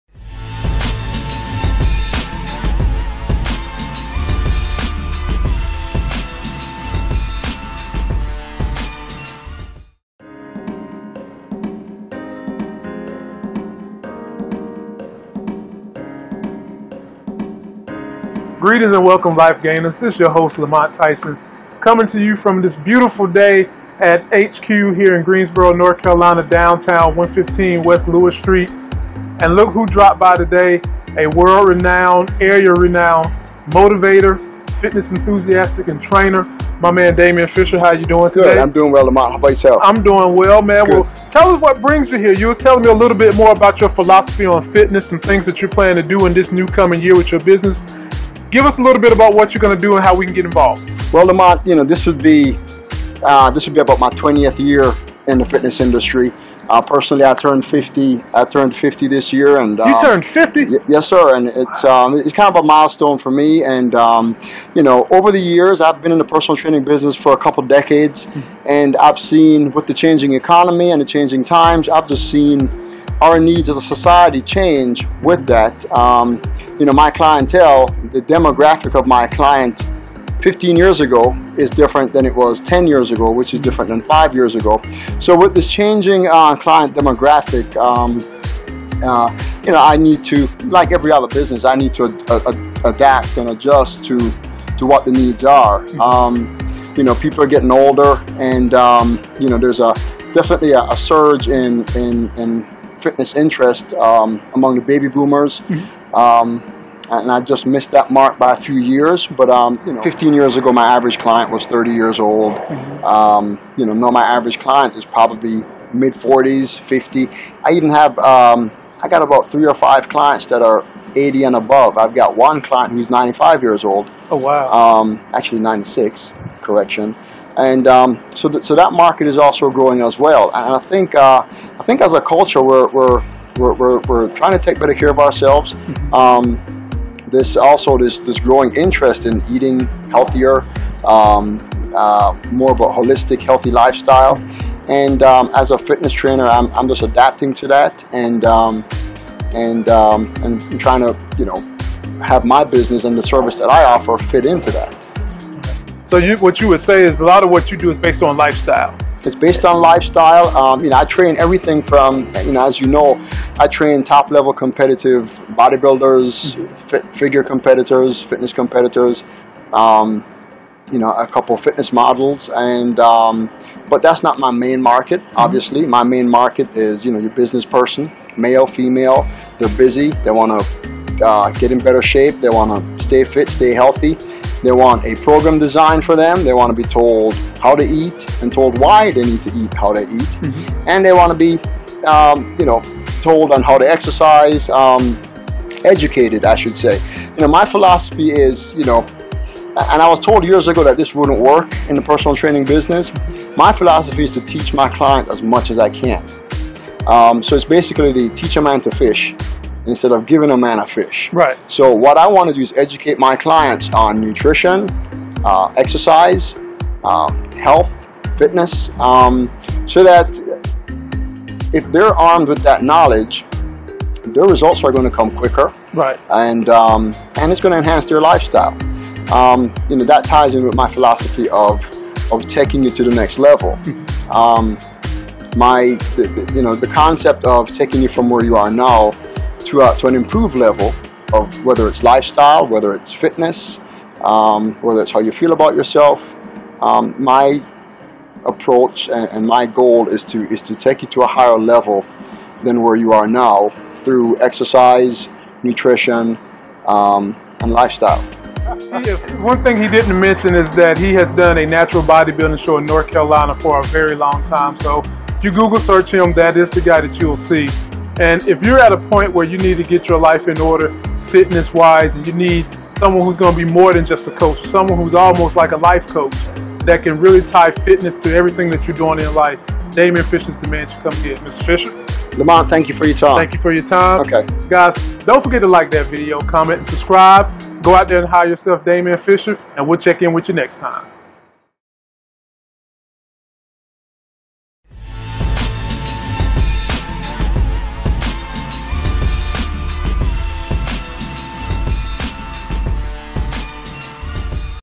Damian Fisher: Rebranding Fitness
See why fitness has been overhauled with Damian Fisher.
Understand his philosophy on health and fitness.

Renfit.com